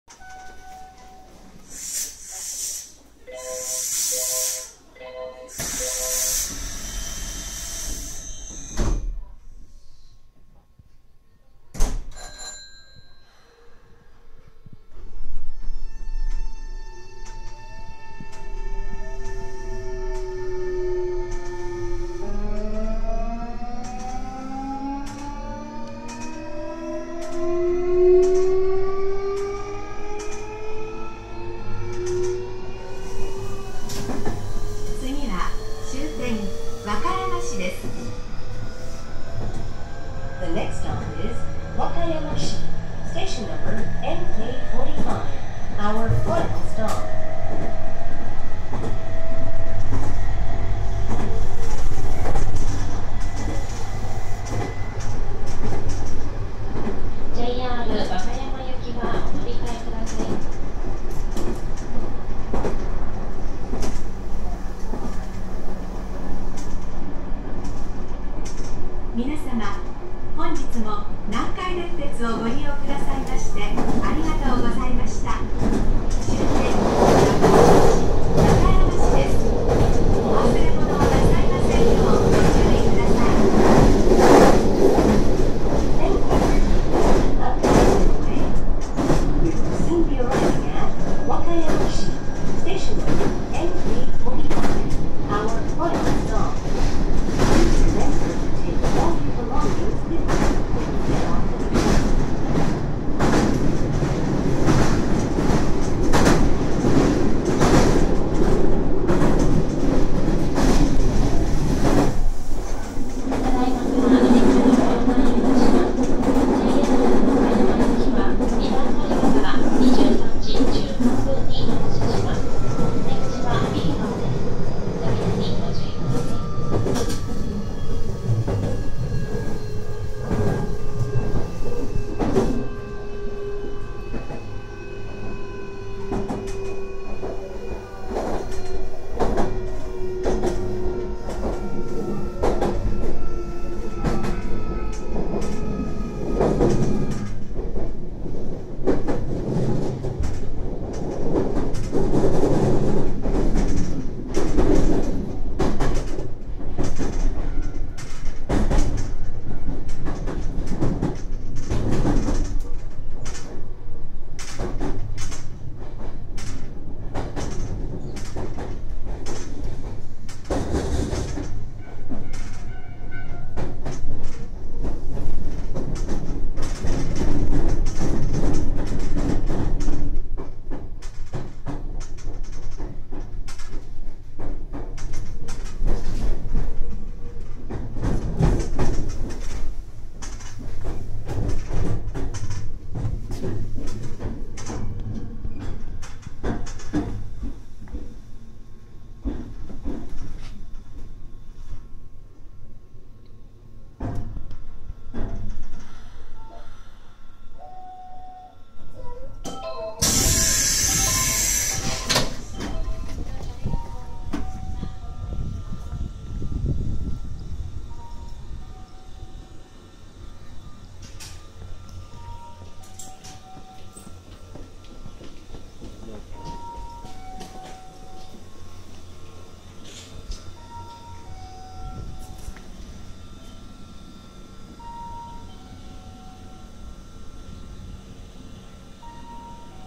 2015年10月14日(水)・南海8300系 8301F(和歌山市到着)【字幕あり】
8301F
普通車・和歌山市行き(6897レ)
22:31・下り和歌山市到着